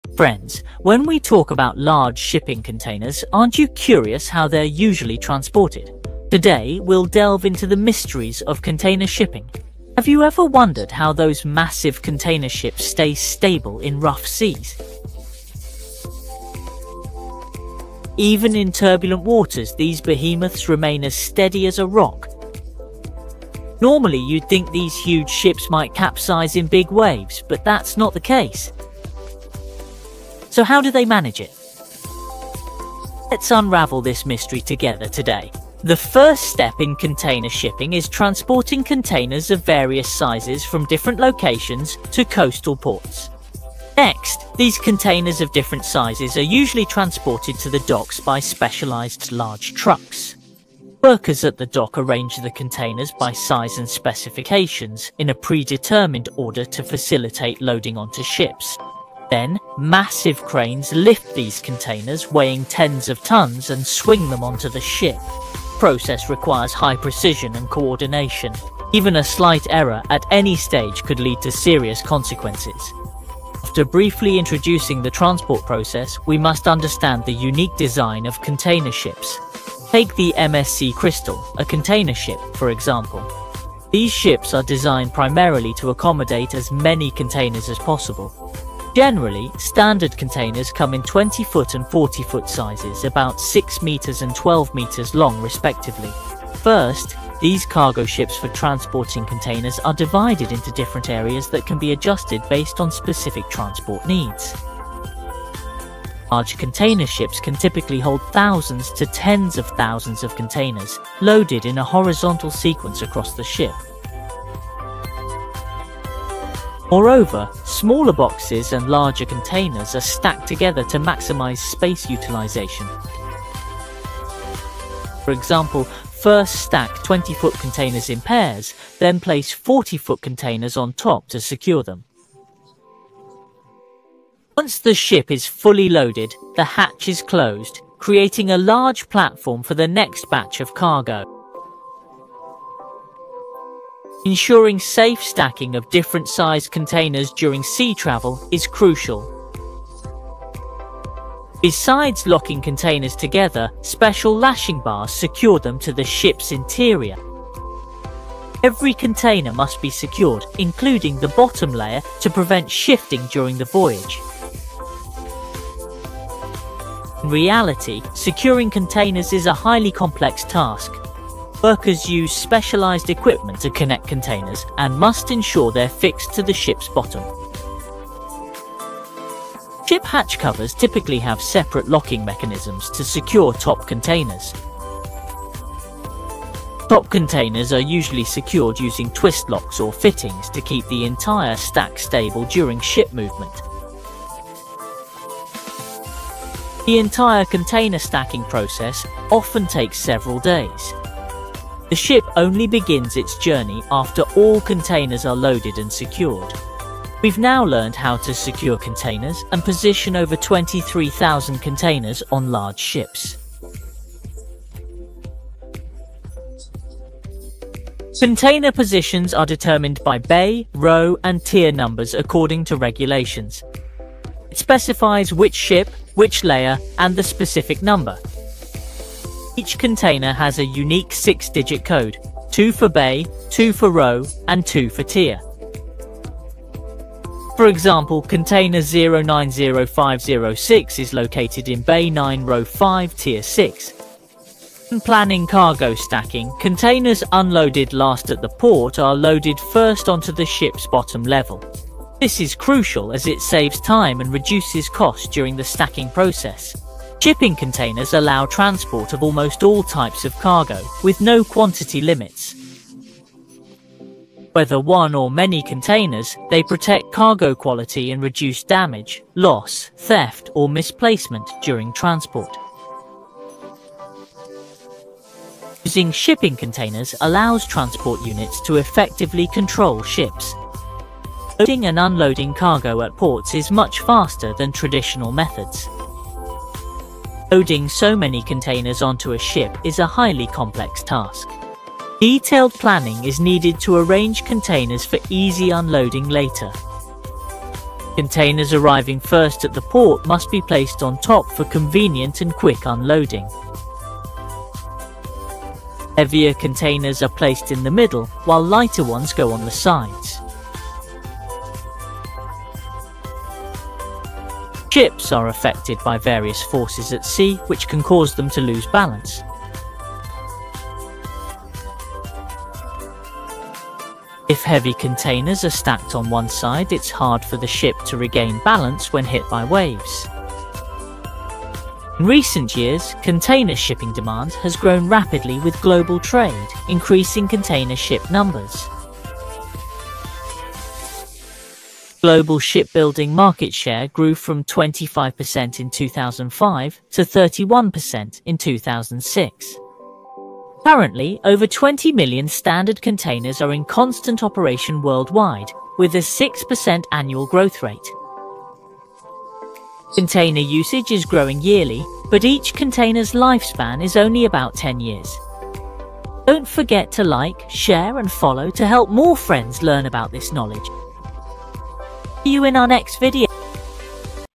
Jaw-Dropping Container Ship Stacking: How 23,000 Containers Reach 40 Meters High!
Join me in this mind-blowing video as I reveal the incredible process behind stacking 23,000 containers on a giant cargo ship that reaches a staggering height of 40 meters! Witness the stunning logistics, engineering secrets, and powerful machinery that make this feat possible. Discover how each container is carefully placed to ensure safety, stability, and efficiency on the world’s largest ocean vessels. This exclusive behind-the-scenes look will amaze shipping fans, logistics professionals, and tech enthusiasts alike. Don’t miss the video—watch now and unlock all the incredible details that power global trade!